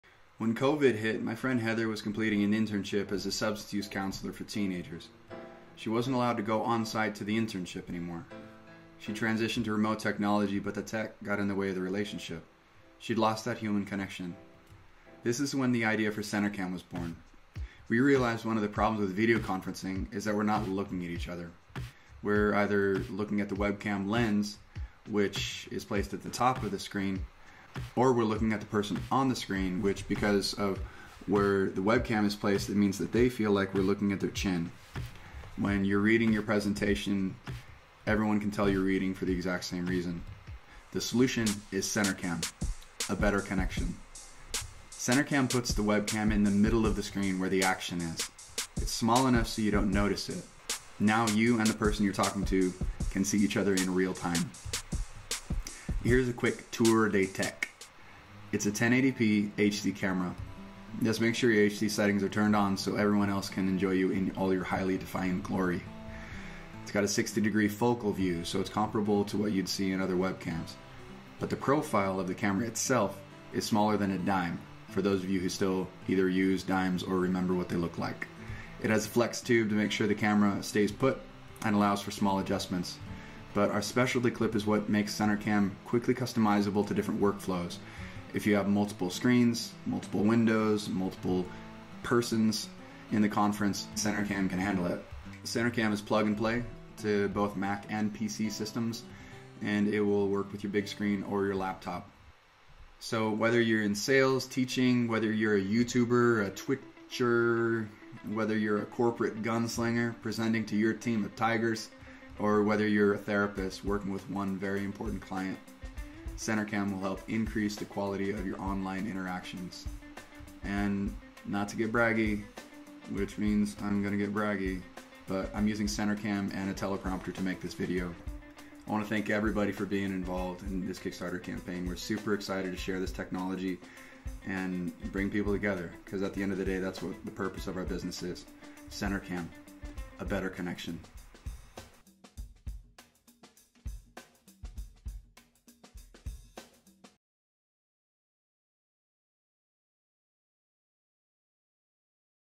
Center Cam: a revolutionary new middle-screen webcam
It's finally here. A webcam that allows for eye contact during videoconferencing. Center Cam is a revolutionary, ie, SIMPLE solution to a major problem. A problem so pervasive, many of us don't even know its a problem. We've been stuck with screen-edge webcams, and now there is an alternative. Center Cam: A Better Connection! Coming to Kickstarter, January 15, 2021.